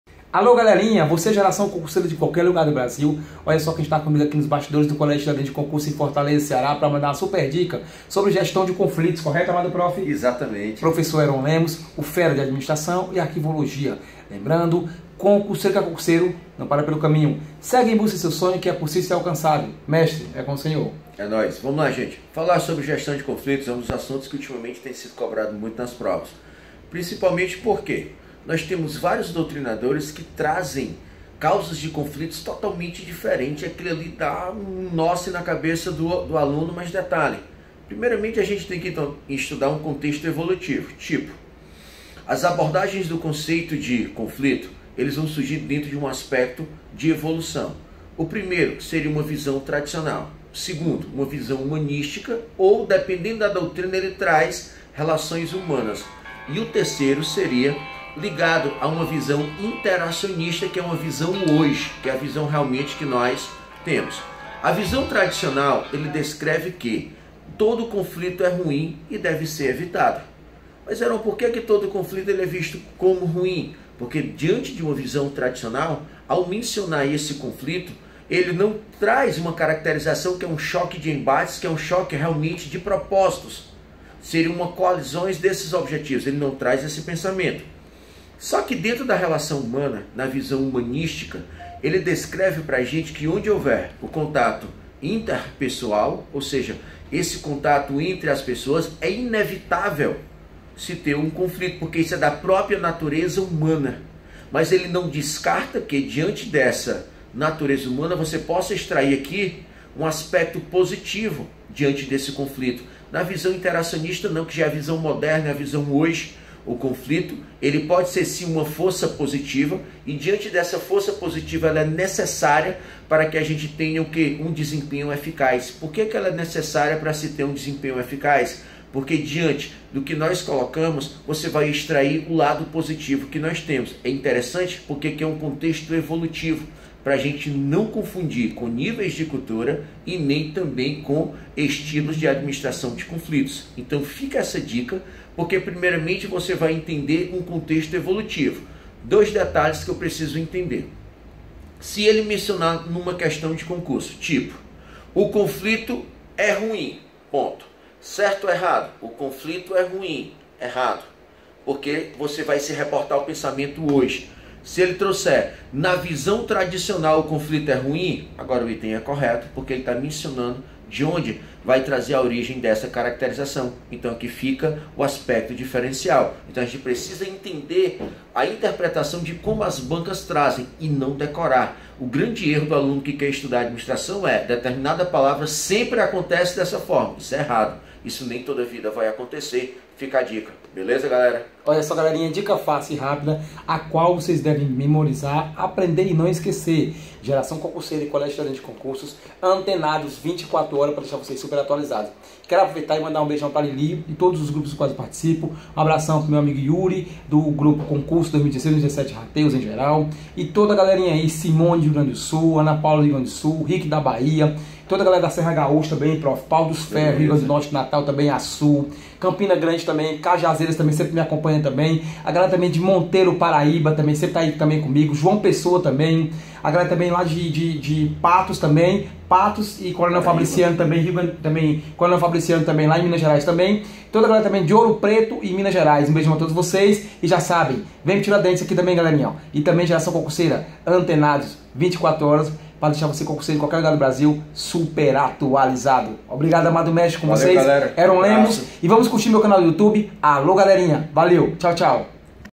Administração com o professor Heron, Assunto Gestão de Conflitos.
Administração com o professor Heron Lemos, Assunto: Gestão de Conflitos.
Curtam o meu canal do YouTube.
Obrigado a todos valeu Galerinha.